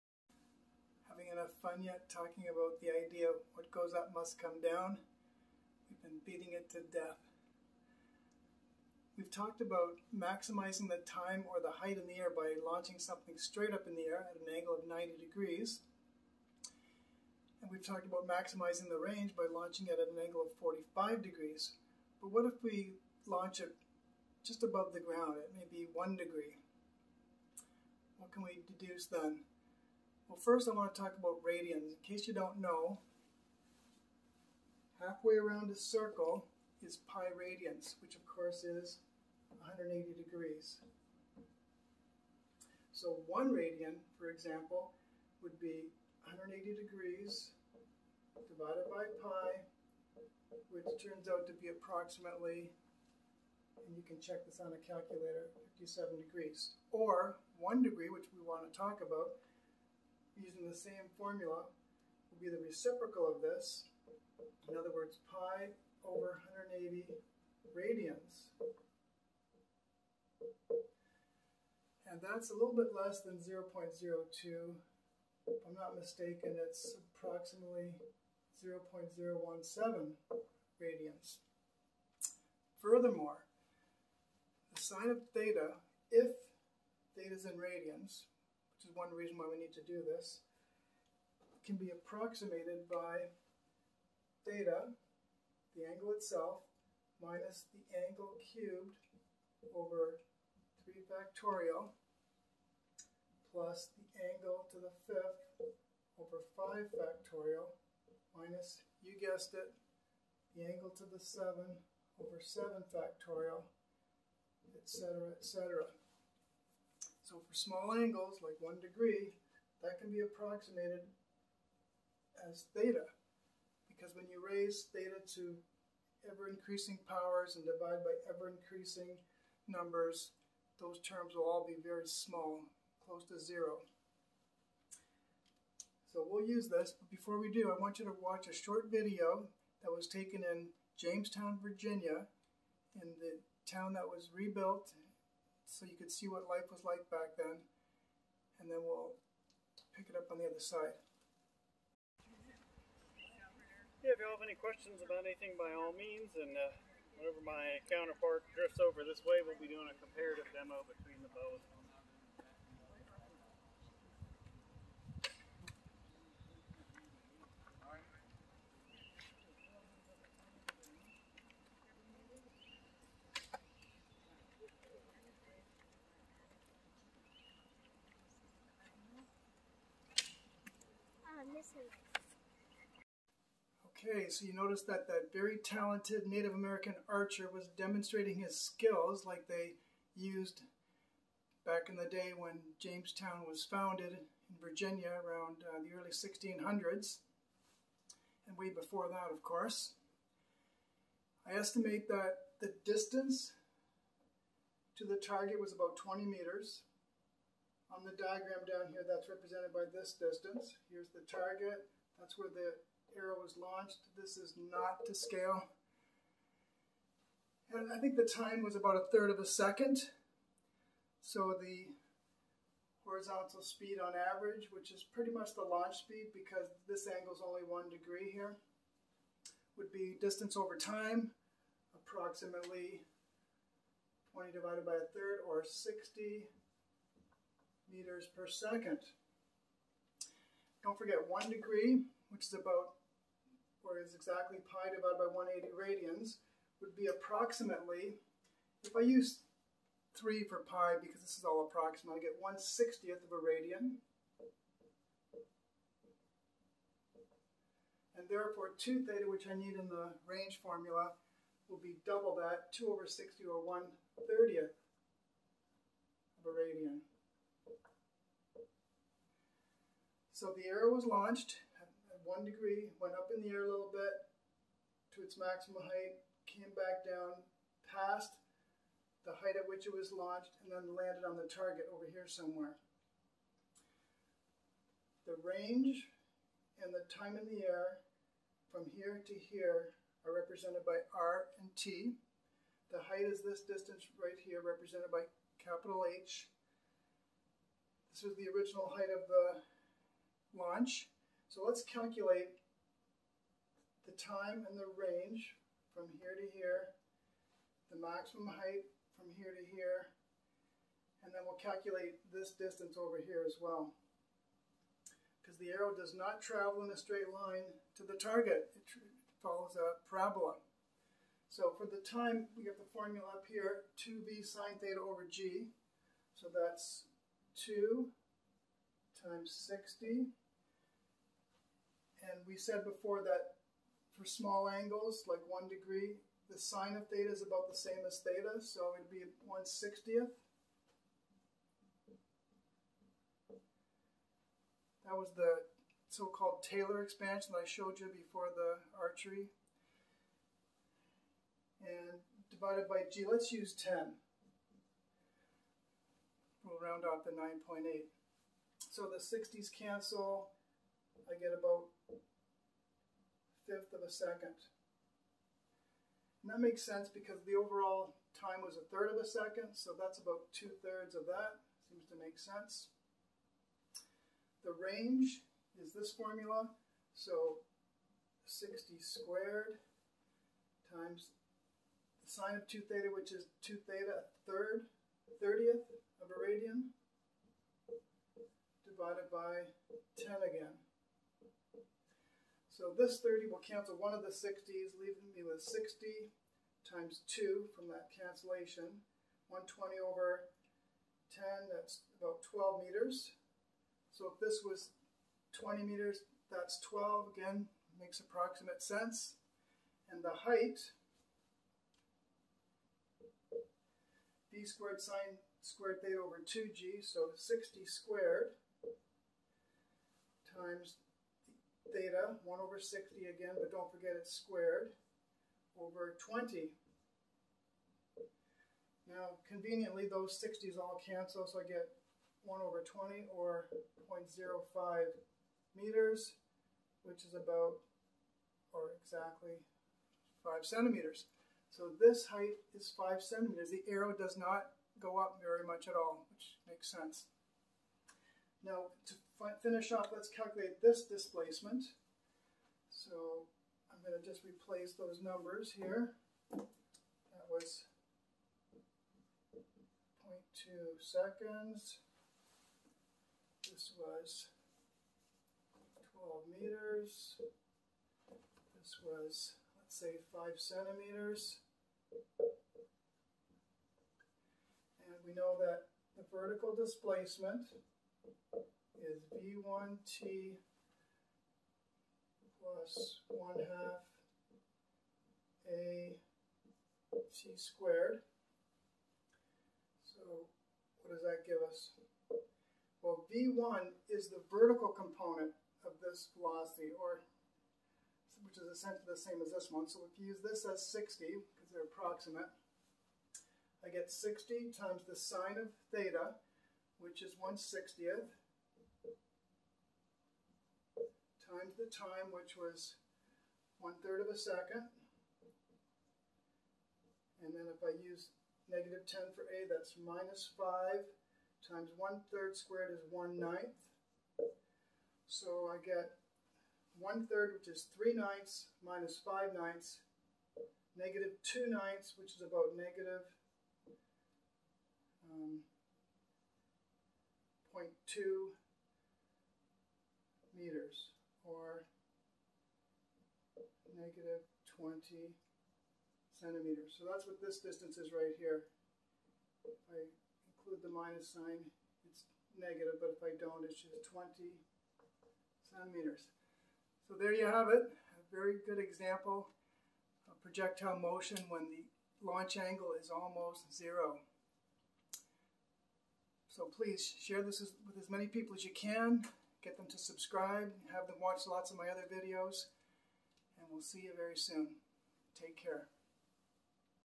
Projectile Motion, part 8: an Arrow
Mr. John Hush calculates the trajectory of an arrow shot by a native American archer in the rebuilt village of Jamestown, Virginia, USA.